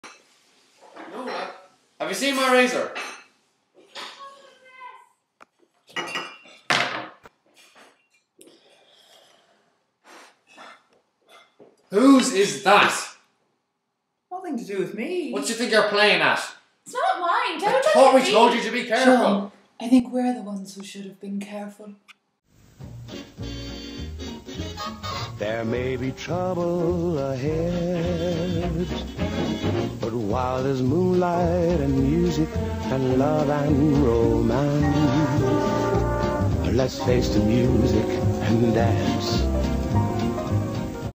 Trouble Ahead
An Irish re-edit of the Allied Dunbar ad from the 90s.